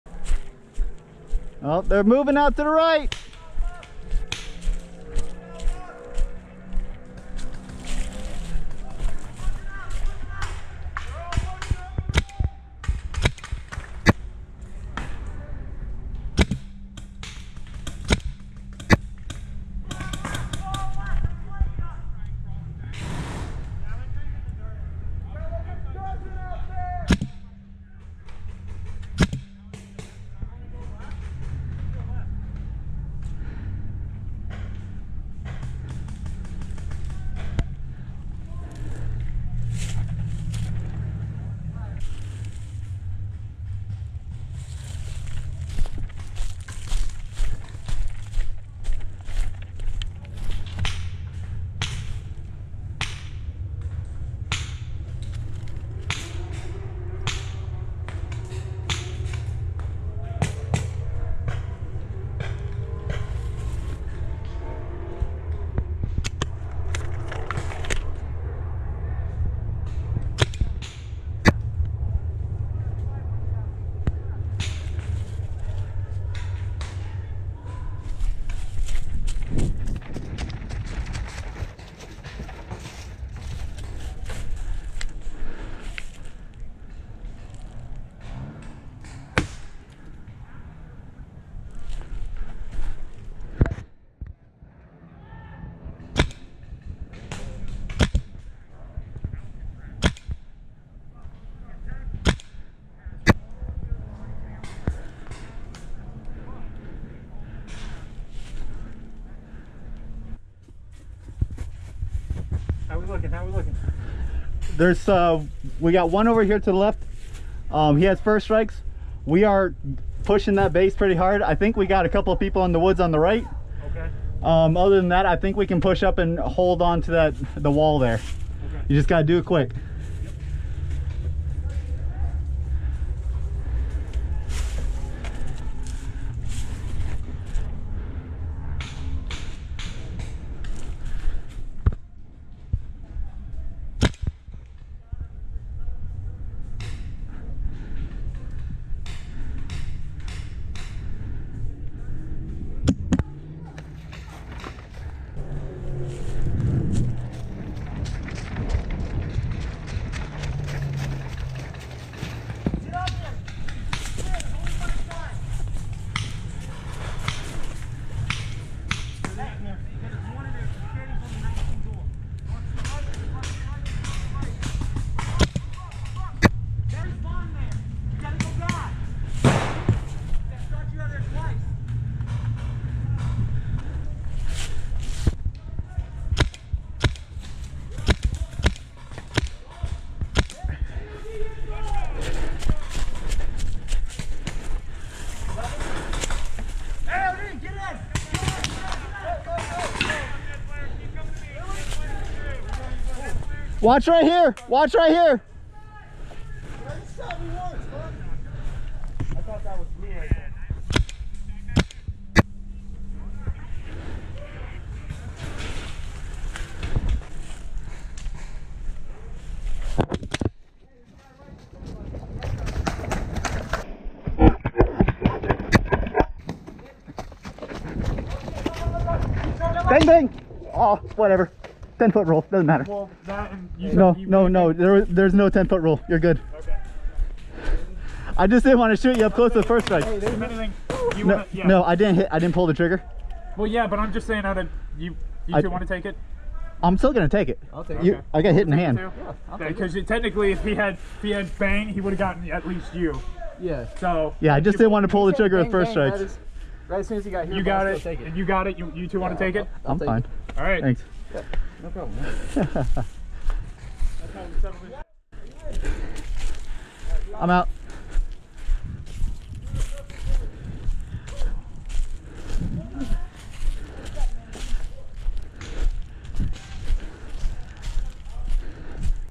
Magfed Florida MFOG at BFO Pt 2 - Planet Eclipse MG100 Gameplay (EMF100)
Magfed Florida MFOG at my home field, Battlefield Orlando!  This was a FSR friendly game with a bunch of players.  The opposing team had unlimited lives and we had a limited number of lives for our team.  Our goal was to capture and hold their base for 2 min in order to push them back to their final defensive position to win.

Picture courtesy of Jay Franco.

My equipment:

First Strike 26/3000 Tank

My Cameras:

Gopro Hero 6
Scope Cam